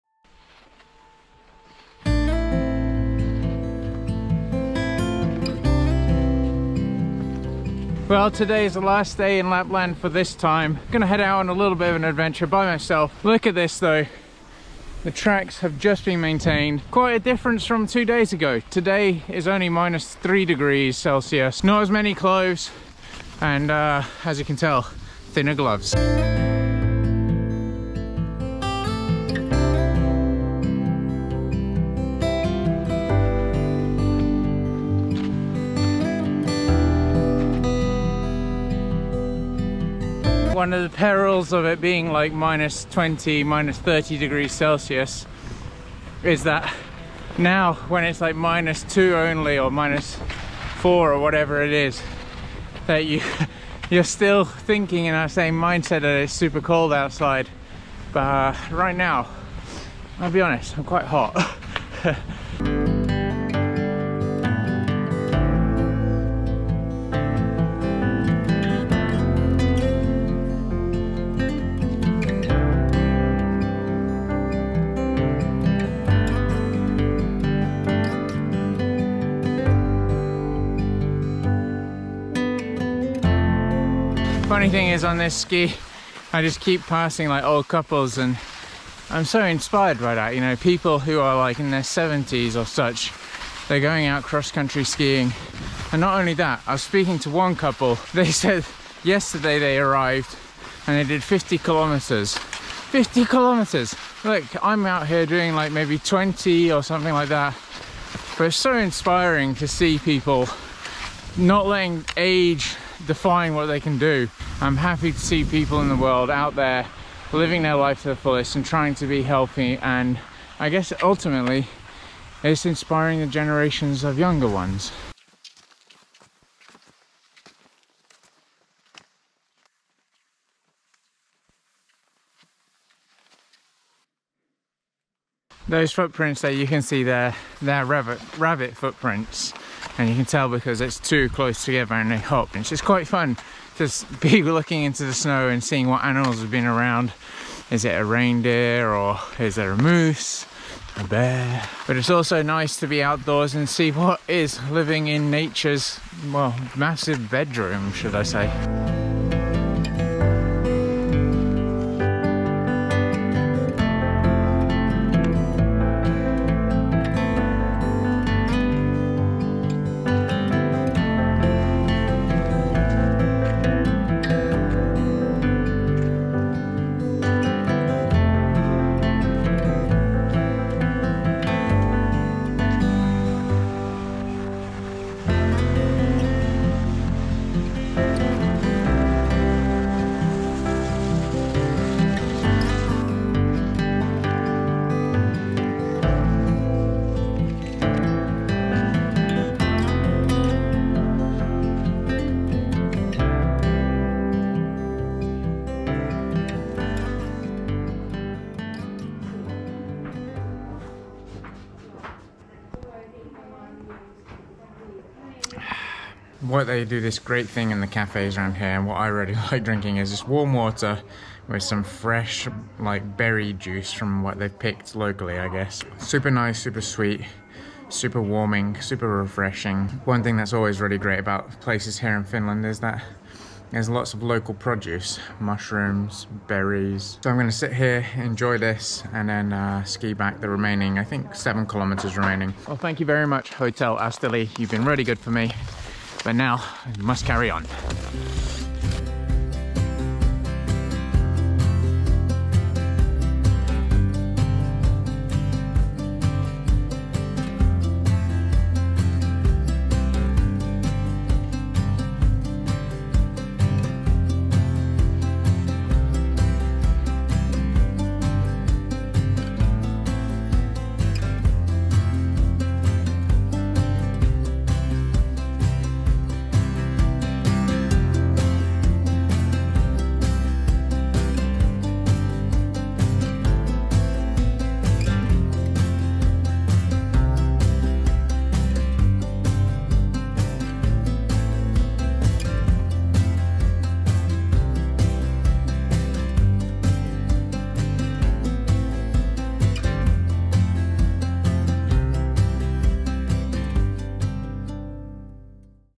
Alone in Lapland...
Well my trip to lapland comes to an end with a great 30km ski in Finnish Lapland